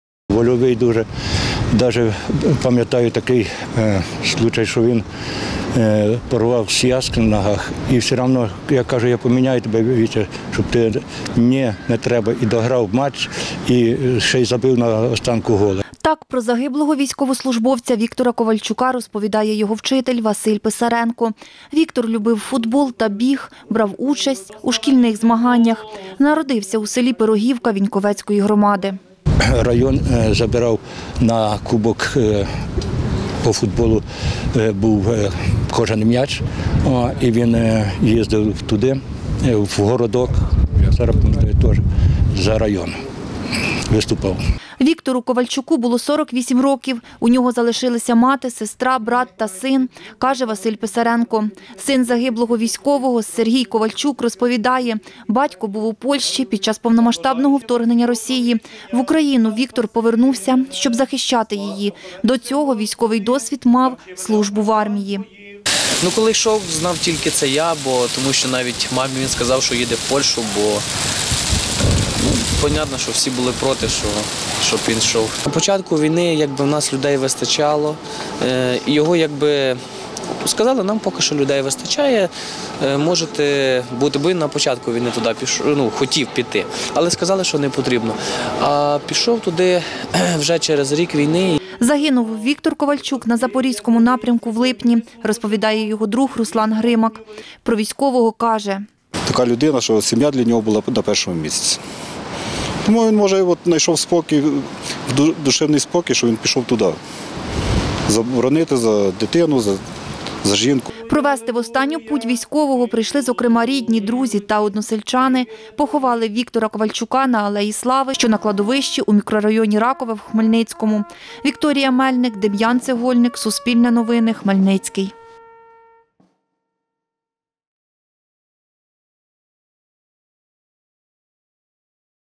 У Хмельницькому попрощались з Віктором Ковальчуком
У Хмельницькому попрощалися із 48-річним військовим Віктором Ковальчуком. Він був родом із села Пирогівка Віньковецької громади. Під час повномасштабного вторгнення приїхав із-за кордону та став на захист України.